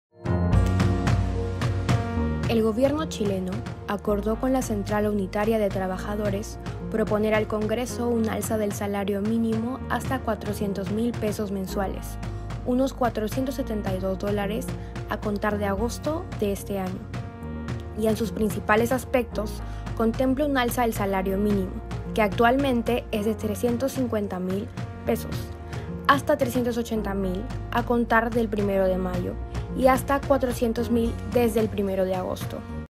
Sueldo mínimo en Chile a 472 dólares
Sueldo mínimo en Chile espera superar los 470 dólares.